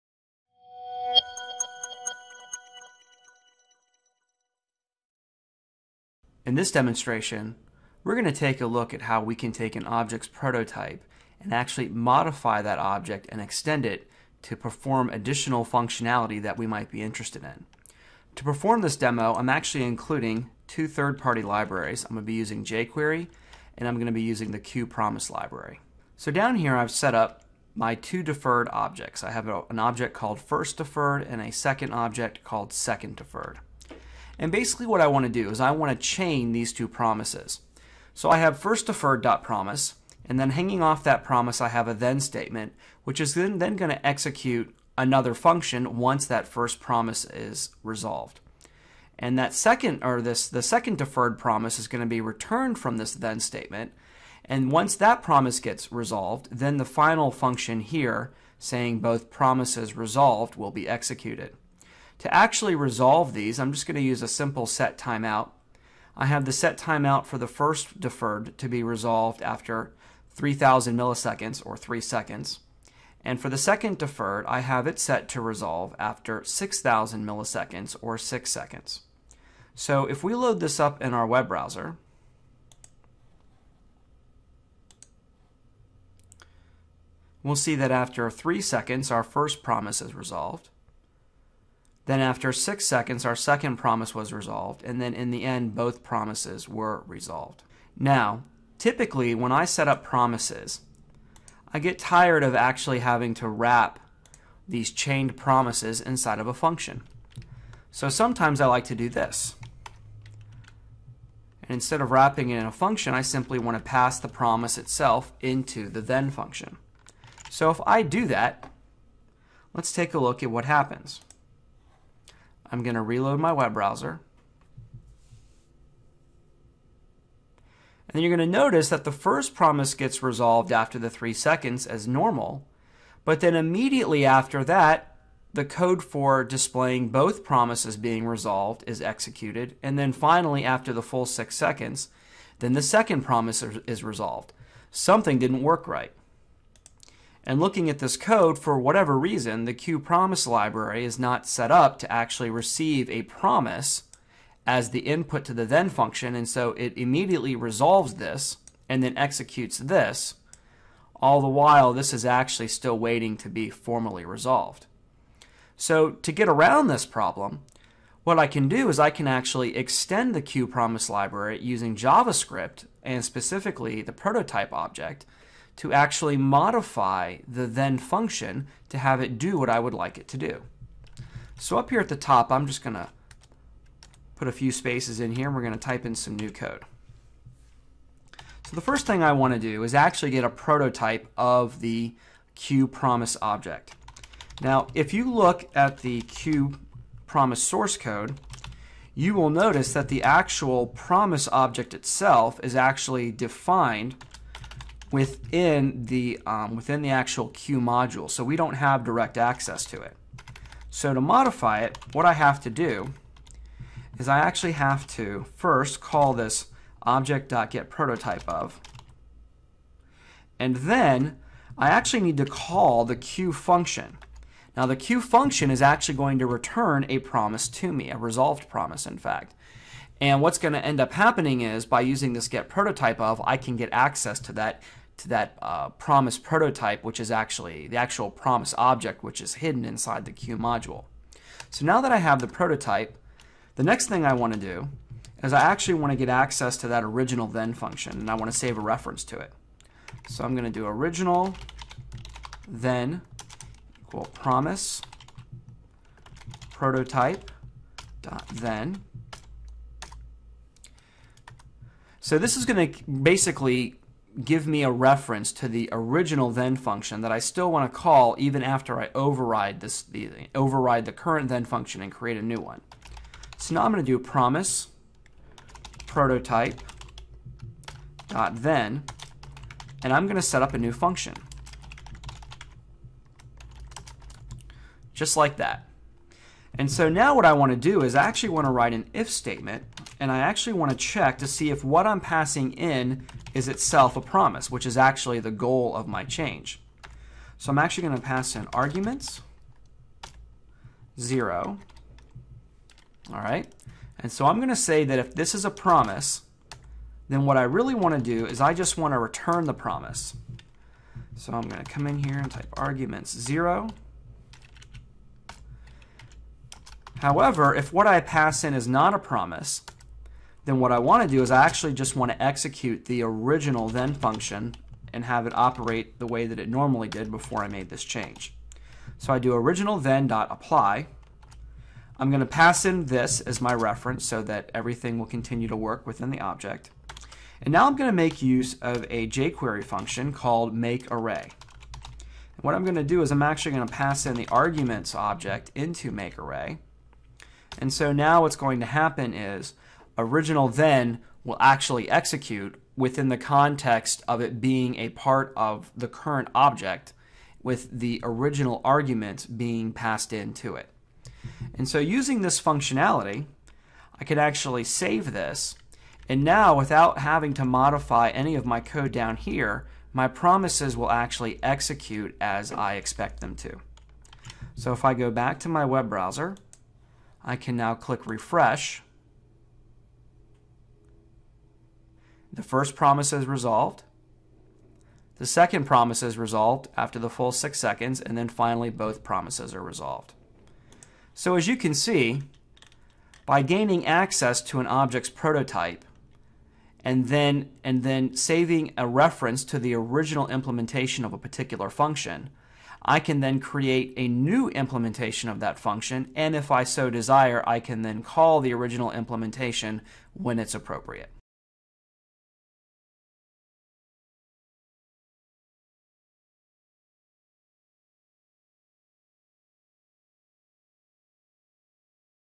JavaScript Video Tutorial - Extending a Prototype Object and Modifying It
An example of how you can extract a prototype of an object and then replace the prototype's methods with new custom methods.

For a transcription of this video and even more content on engineering and technology, visit us